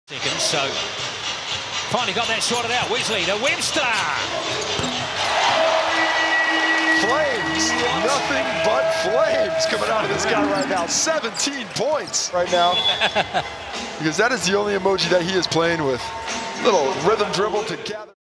Webster flames from 3, SKYCITY Breakers vs Cairns Taipans, Fri 9th Nov
Tickets Are Available Now.